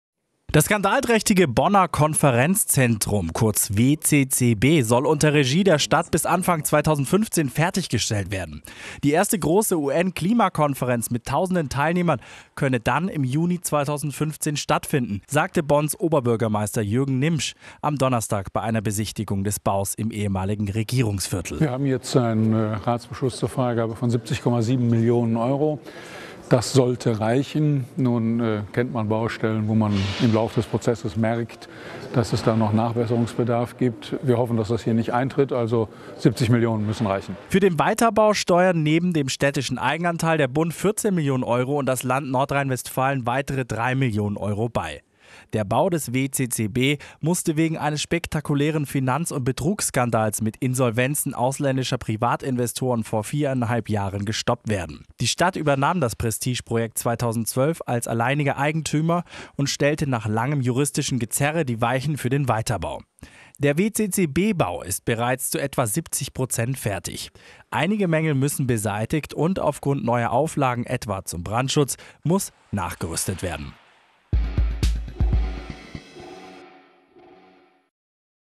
Bonner Konferenzzentrum WCCB wird bis Anfang 2015 fertiggebaut
Endlich: Nach gut vierjährigem Baustopp wird am Bonner Konferenzzentrum WCCB wieder gearbeitet. Anfang 2015 soll es fertig sein und Mitte nächsten Jahres soll die erste große UN-Konferenz stattfinden. - NewsNow - Stets bestens informiert!